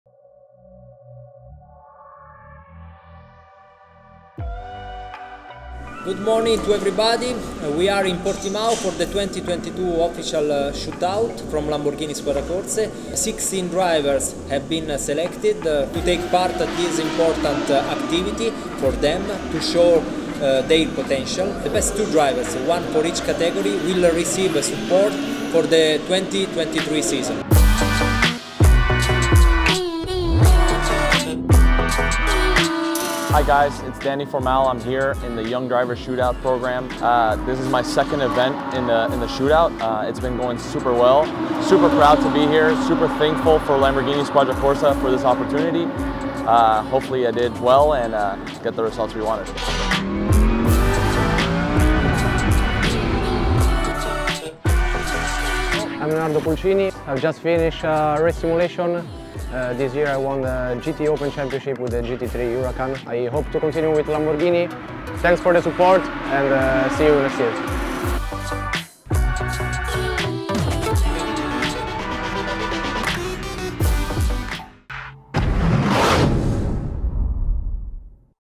Lamborghini Youngster Program 2022
Leonardo Pulcini and Danny Formal have been announced as the stand-out drivers of Lamborghini Squadra Corse’s end of year Youngster Program shootout. Winning the GT3 Junior Program, Pulcini has been promoted to Lamborghini Factory Driver for 2023, while Formal is one of three drivers to graduate to the newly created Young Professional Driver category.